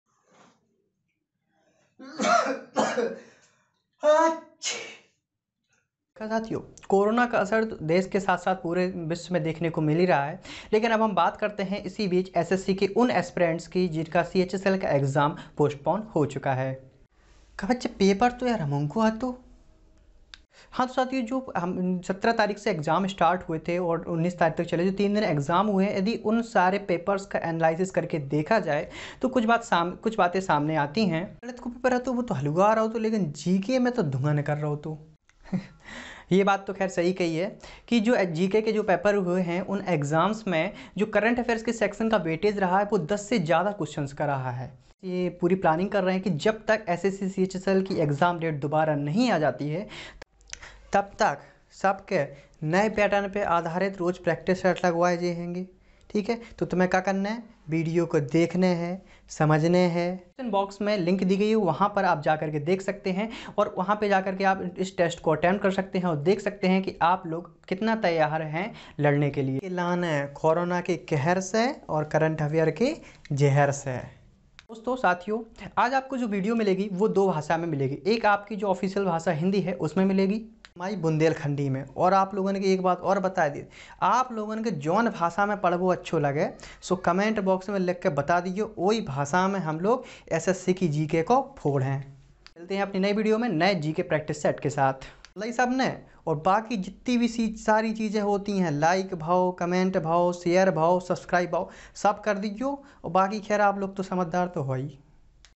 ANNOUNCEMENT (EXAM POSTPONNED के बाद की तैयारी) CHSL GK TEST SERIES
हम  SSC CHSL की प्रैक्टिस फिर से  प्रारंभ कर रहे हैं / ये सीरीज पिछली  परीक्षाओं का अध्ययन कर  तैयार की गयी है तो जब तक EXAM की तारीख दोबारा नहीं आती चलिए अपनी तयारी को और बेहतर बनाते हैं /